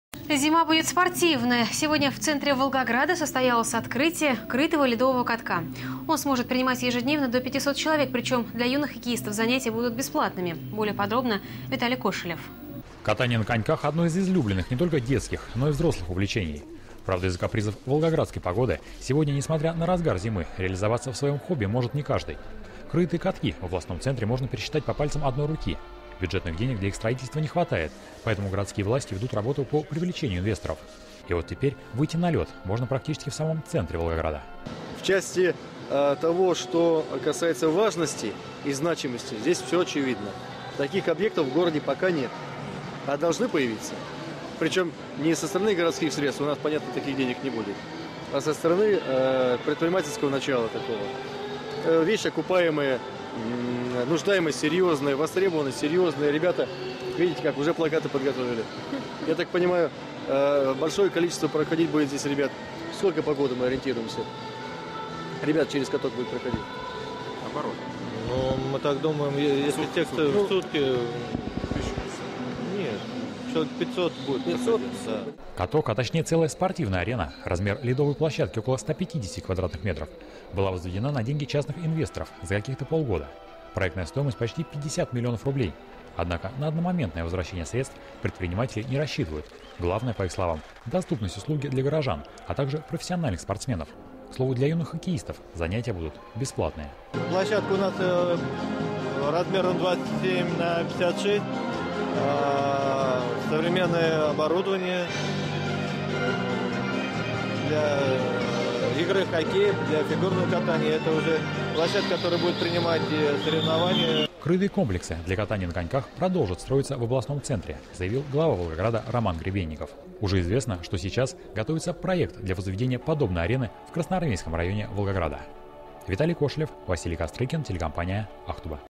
ОТКРЫЛСЯ ЛЕДОВЫЙ КАТОК
Зима будет спортивной. Сегодня в центре Волгограда состоялось открытие крытого ледового катка. Он сможет принимать ежедневно до 500 человек, причем для юных хоккеистов занятия будут бесплатными.